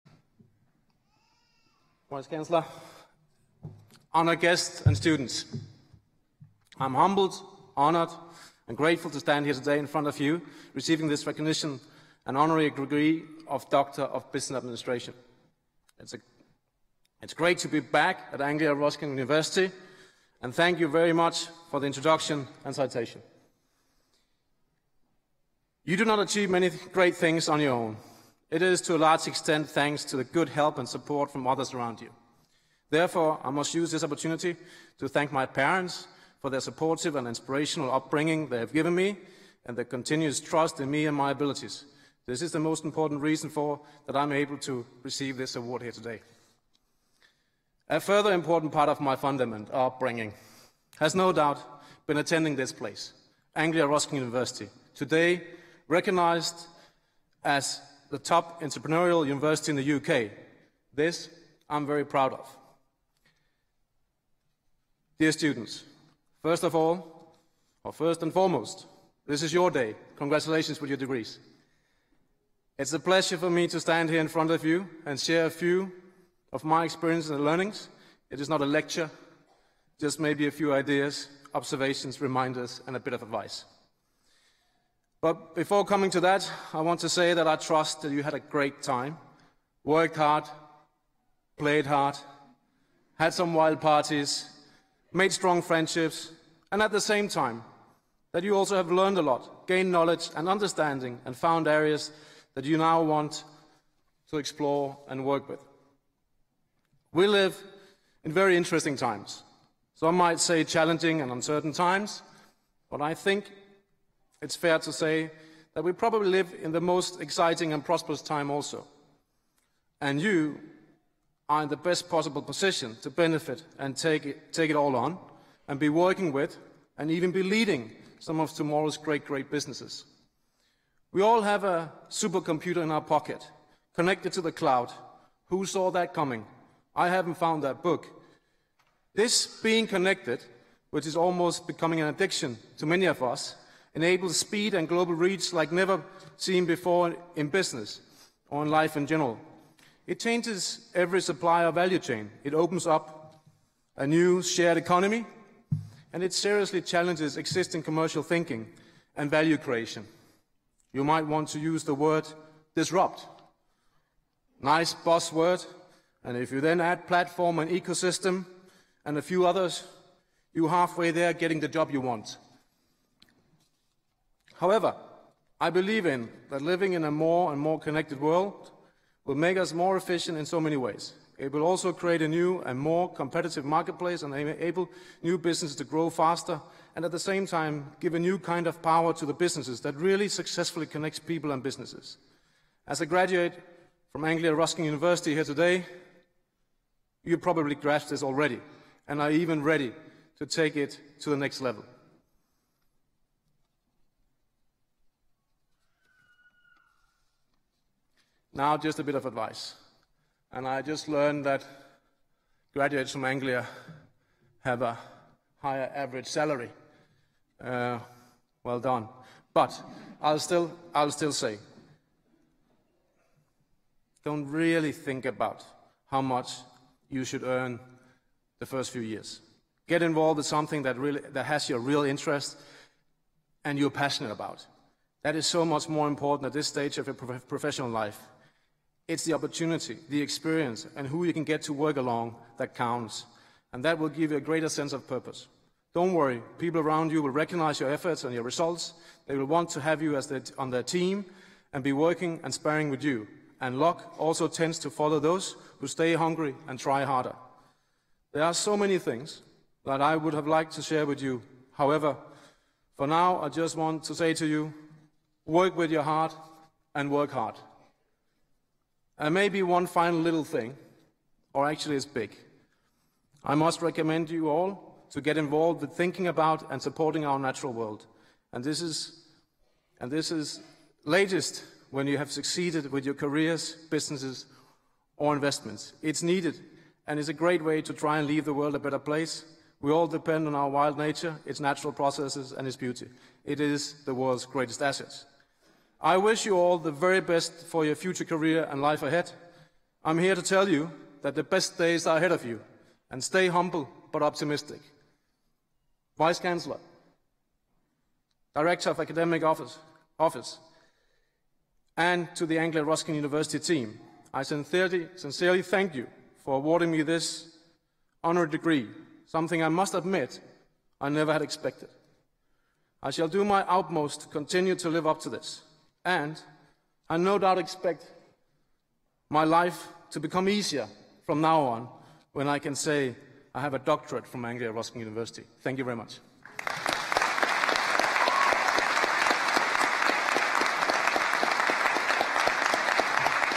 Dr Anders Holch Povlsen
Lord Ashcroft International Business School Graduations
Anglia Ruskin University
Thursday 15 October 2015 - 6PM
Cambridge - Corn Exchange